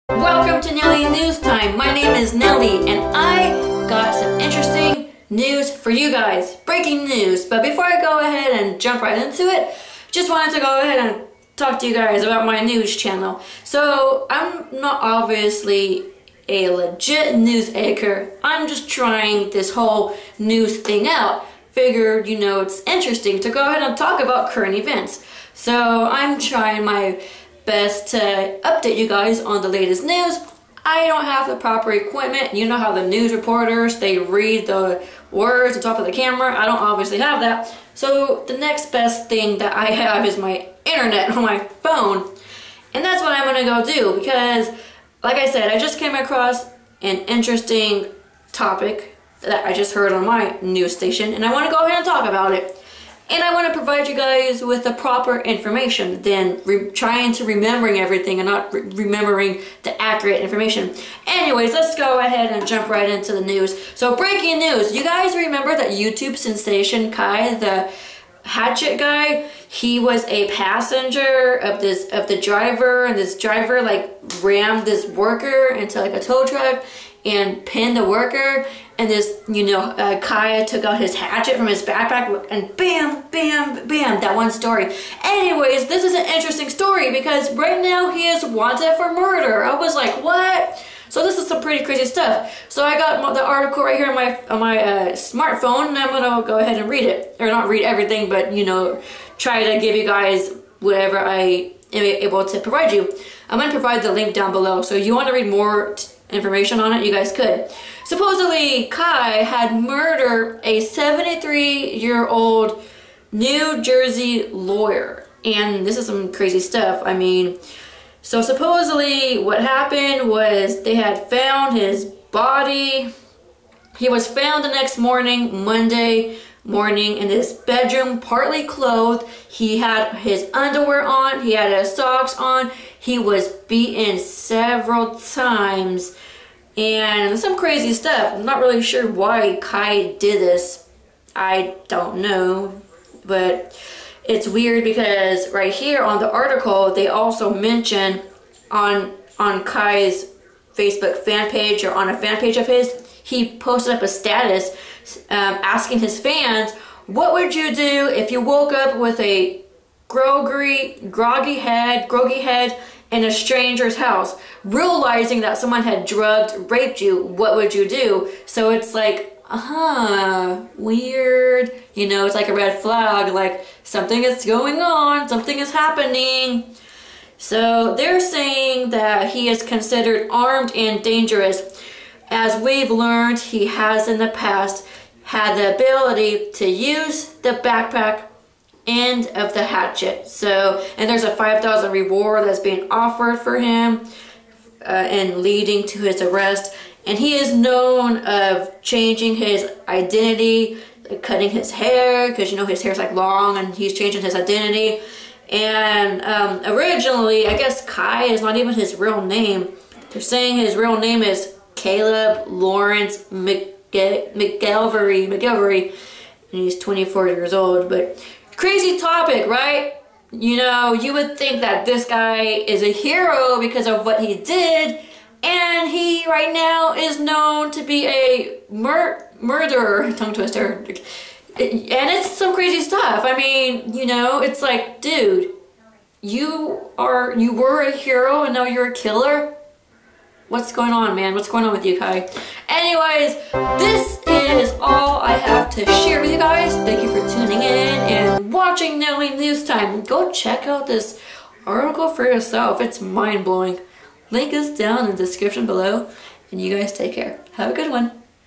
YOUTUBE STAR KAI WANTED FOR NEW JERSEY MURDER!!!!
First A Hero, Now A Murderer. Crazy Stuff!!!

Fresno Hatchet-Wielding YouTube Star 'Kai' Wanted For NJ Murder

Viral star Kai the hatchet-wielding hitchhiker wanted for murder

Breaking News!!!
He Has Been Arrested!!!
Famed 'hatchet hitchhiker' arrested in NJ homicide

Social Networks!

Add Me on xbox 360!!!
GamerTag : GamerChickNelly

Skype Me!
Nellyrocks89

Nellys Tumblr!